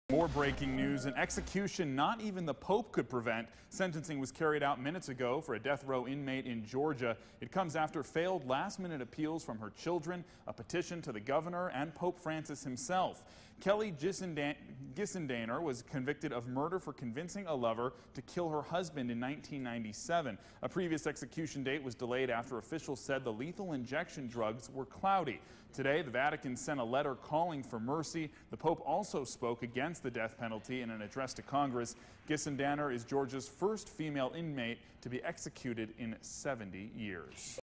Death row inmate dies after appeal from Pope
First female GA inmate executed in 70 years